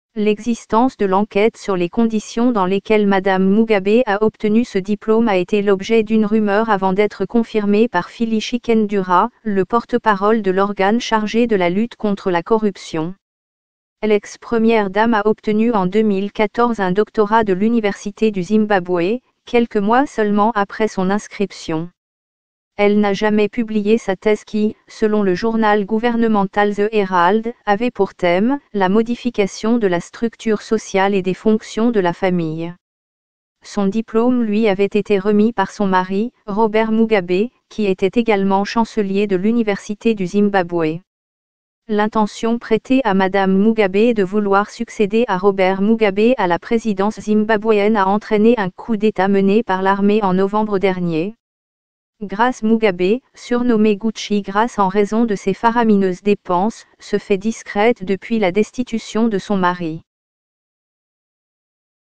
Une enquête sur le doctorat de Grace Mugabe
L'existence de l'enquete sur les conditions dans lesquelles Mme Mugabe a obtenu ce diplome a ete l'objet d'une rumeur avant d'etre confirmee par Phyllis Chikundura, le porte-parole de l'organe charge de la lutte contre la corruption.